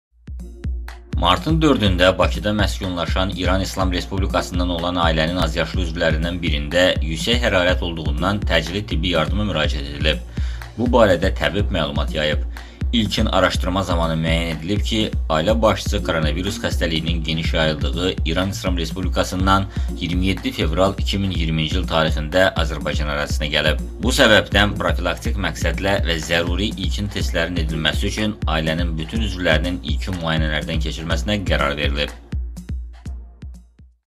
Azərbaycanda səkkiz nəfərlik ailə karantinə alındı
“Martın 4-də Bakıda məskunlaşan İran İslam Respublikasından olan ailənin azyaşlı üzvlərindən birində yüksək hərarət olduğundan təcili tibbi yardıma müraciət edilib”.

Bu barədə TƏBİB məlumat yayıb.

İlkin araşdırma zamanı müəyyən edilib ki, ailə başçısı koronavirus xəstəliyinin geniş yayıldığı İran İslam Respublikasından 27 fevral 2020-ci il tarixində Azərbaycan ərazisinə gəlib. Bu səbəbdən profilaktik məqsədlə və zəruri ilkin testlərin edilməsi üçün ailənin bütün üzvlərinin ilkin müayinələrdən keçməsinə qərar verilib.

Təcili tibbi yardım maşını ilə ailənin digər üzvlərinin (altısı uşaq olmaqla, səkkiz İran vətəndaşı) də xəstəxanaya aparılması məqsədəmüvafiq hesab olunub. Bu zaman onlar həkim briqadasına müqavimət göstərdikləri üçün hadisə yerinə polis əməkdaşları çağırılıb.

Hazırda müəyyən olunmuş karantin müddətinə uyğun olaraq bütün ailə üzvləri xəstəxanada müşahidə altında saxlanılırlar.